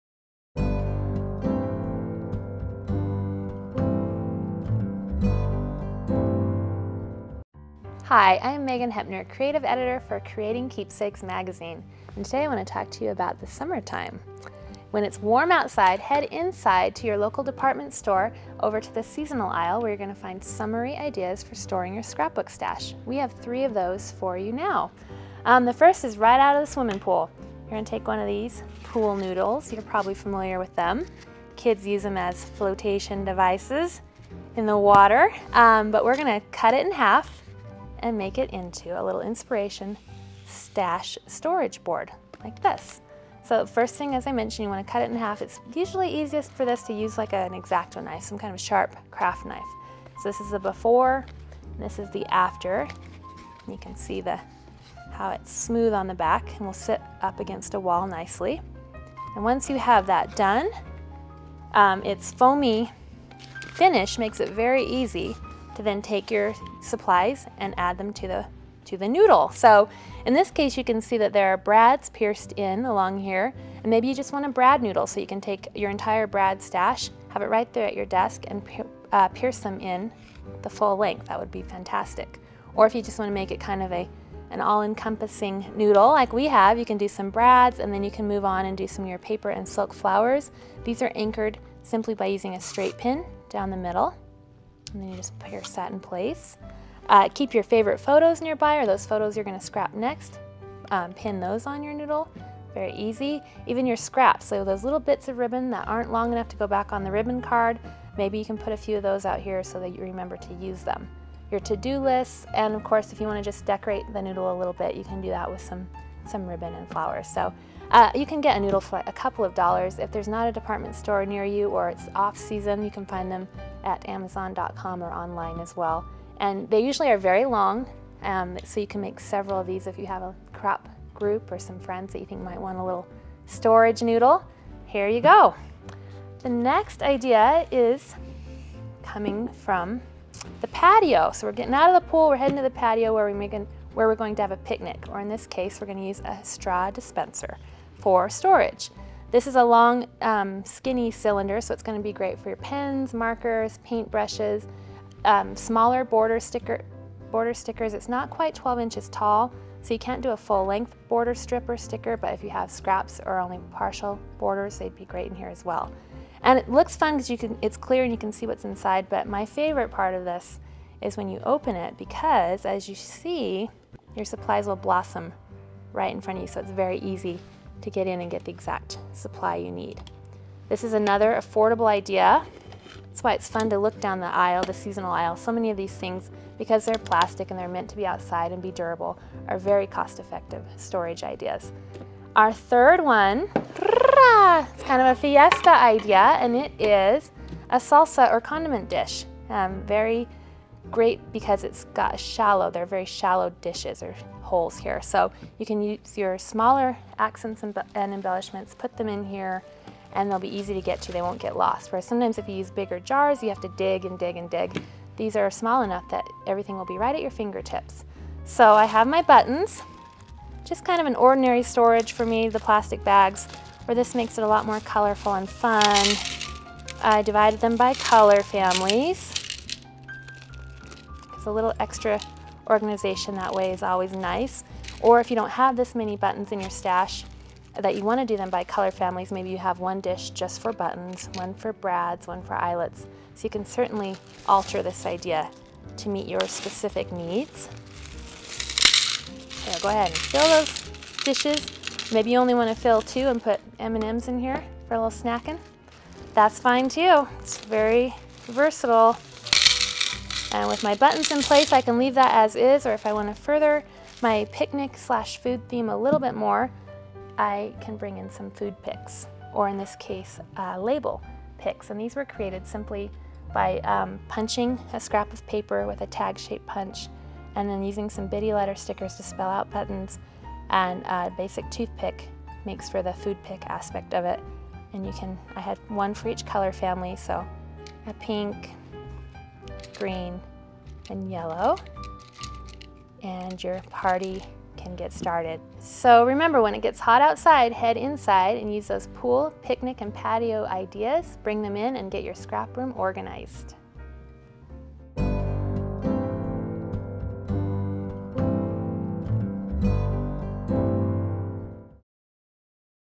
3 Summery Ways to Store Your scrap Stash.m4v
Creating Keepsakes scrapbook magazine shows you three creative ways to store your scrapbooking products using summer-related items.